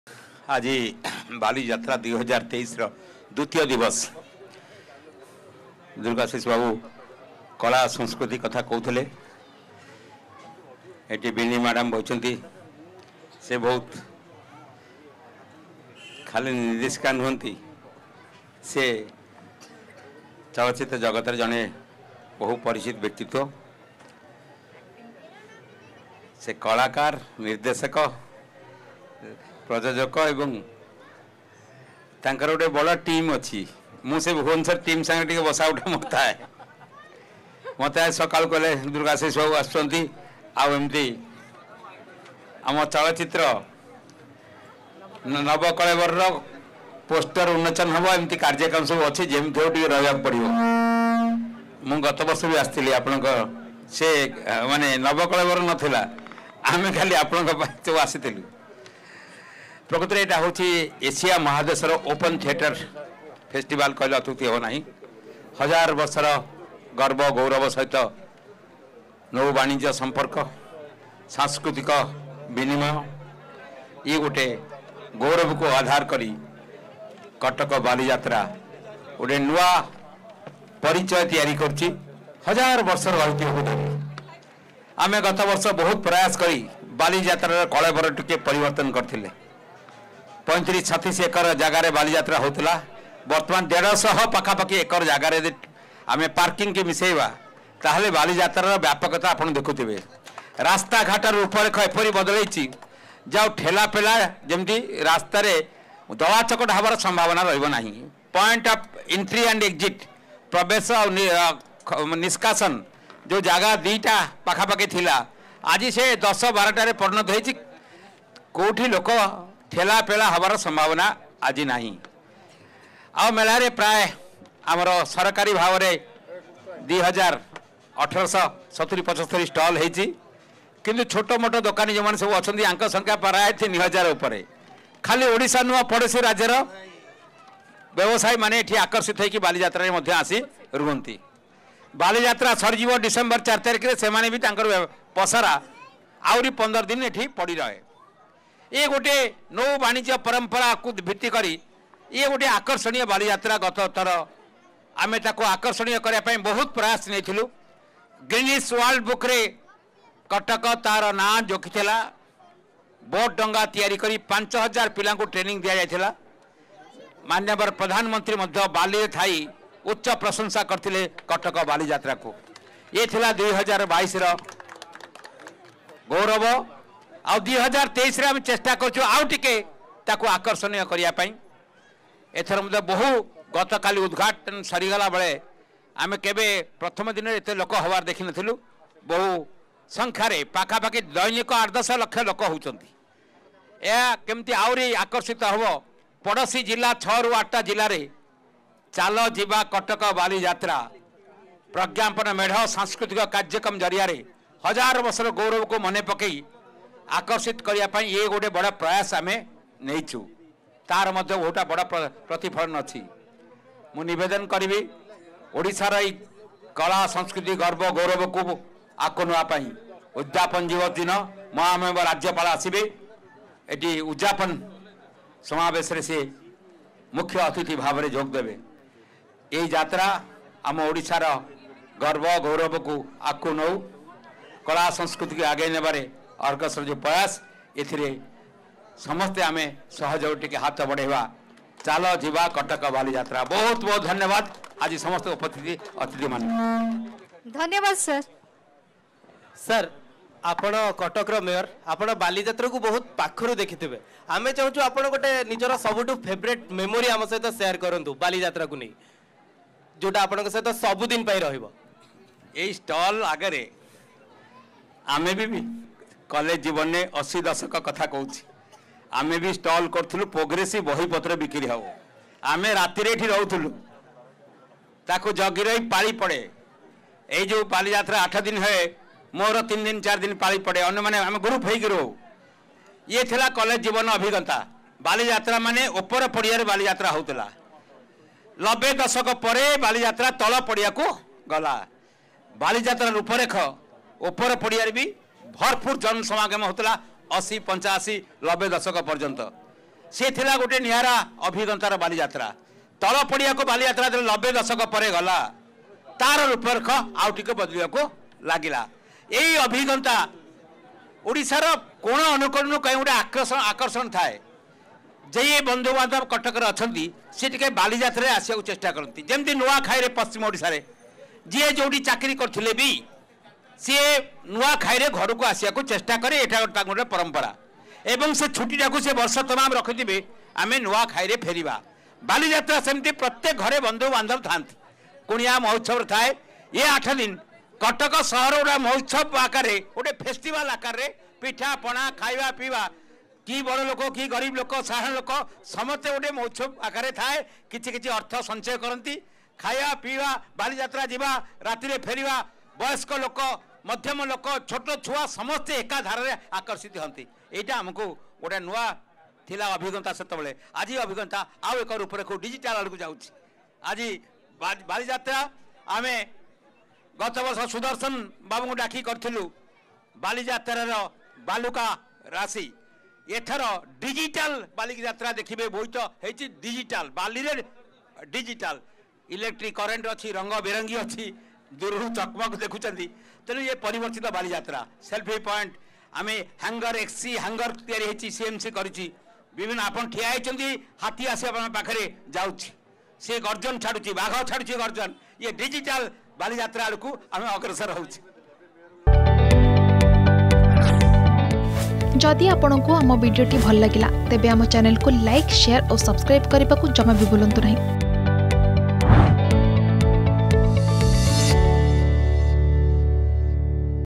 Now Bali Yatra Is Happening In 150 Acres Area : Cuttack Mayor Subhash Singh
ବର୍ତ୍ତମାନ ୧୫୦ ଏକର ଜାଗାରେ ବାଲିଯାତ୍ରା ହେଉଛି || Bali Jatra 2023 || Subhash Singh